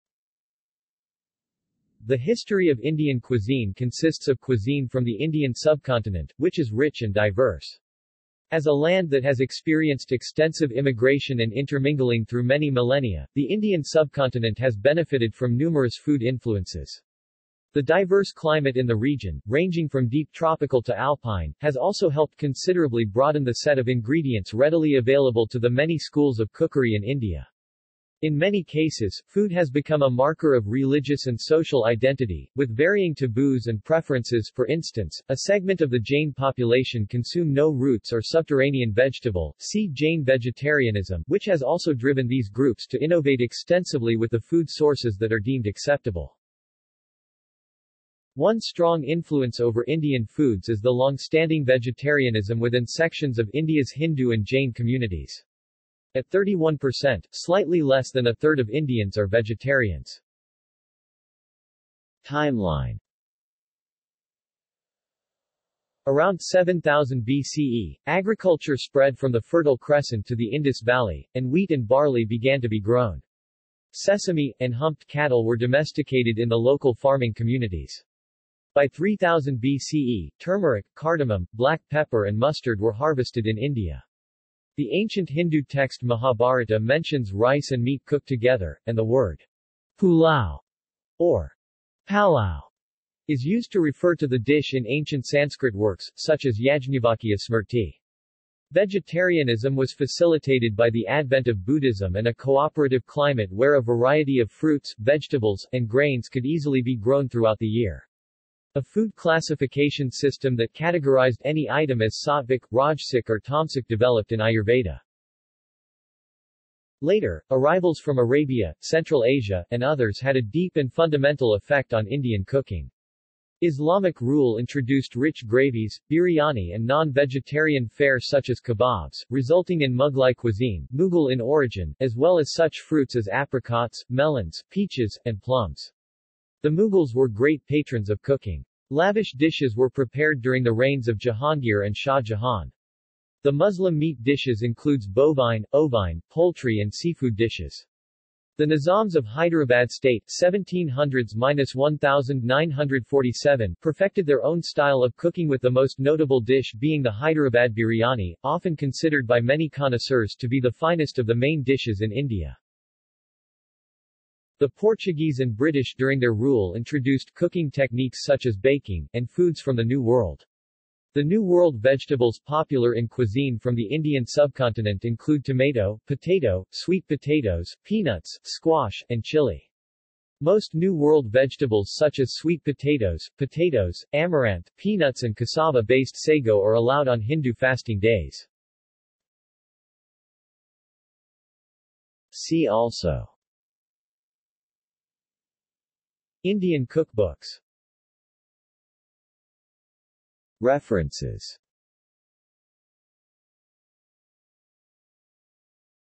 History of cuisine from the Indian subcontinent
The history of Indian cuisine consists of cuisine from the Indian subcontinent, which is rich and diverse. As a land that has experienced extensive immigration and intermingling through many millennia, the Indian subcontinent has benefited from numerous food influences. The diverse climate in the region, ranging from deep tropical to alpine, has also helped considerably broaden the set of ingredients readily available to the many schools of cookery in India. In many cases, food has become a marker of religious and social identity, with varying taboos and preferences (for instance, a segment of the Jain population consume no roots or subterranean vegetable; see Jain vegetarianism) which has also driven these groups to innovate extensively with the food sources that are deemed acceptable.
One strong influence over Indian foods is the longstanding vegetarianism within sections of India's Hindu and Jain communities. At 31%, slightly less than a third of Indians are vegetarians.